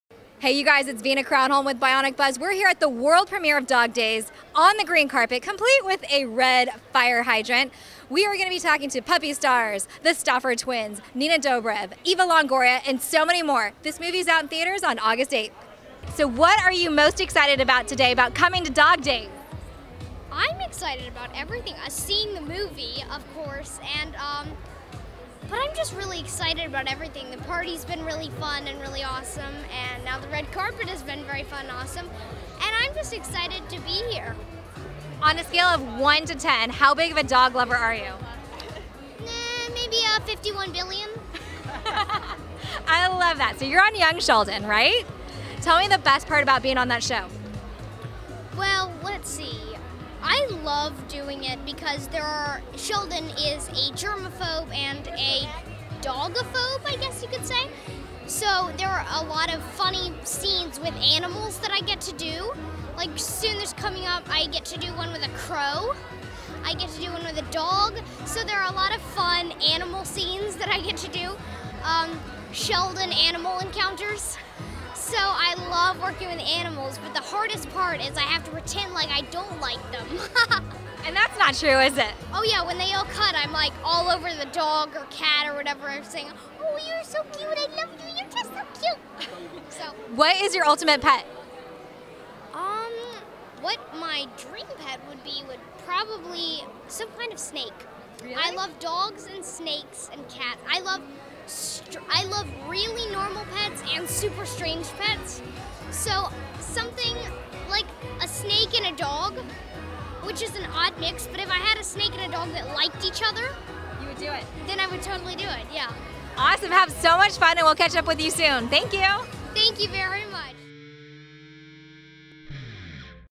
Iain Armitage Interview at Dog Days World Premiere
Bionic Buzz® got to cover the world premiere of the green carpet of Dog Days.The movie follows a group of interconnected people in Los Angeles who are brought together by their lovable canine counterparts. The film is scheduled to be released on August 8, 2018, by LD Entertainment.

On the green carpet we got to interview special guest actor Iain Armitage who stars in Young Sheldon.